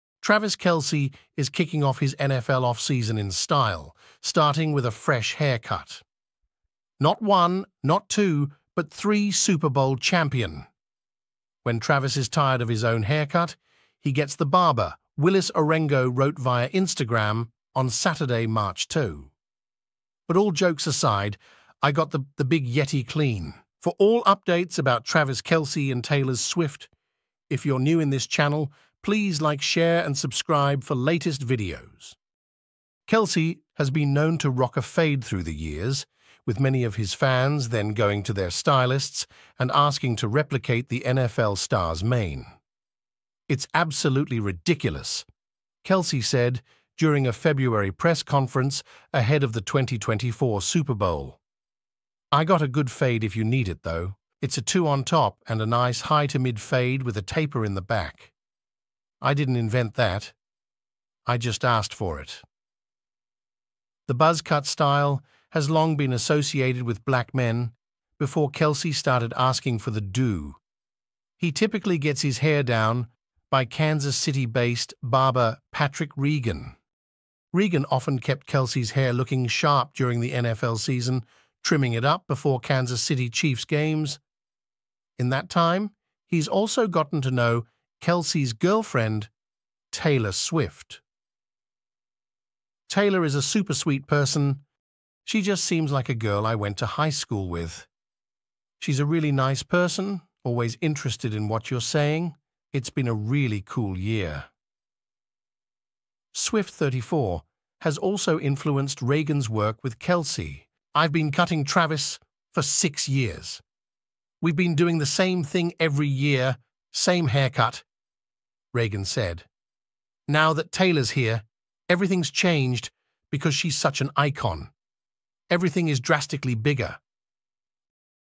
Travis Kelce Gets ‘Clean’ Haircut in Philadelphia After Super Bowl Championship#trending#viralvideo
Travis Kelce Gets ‘Clean’ Haircut in Philadelphia After Super Bowl Championship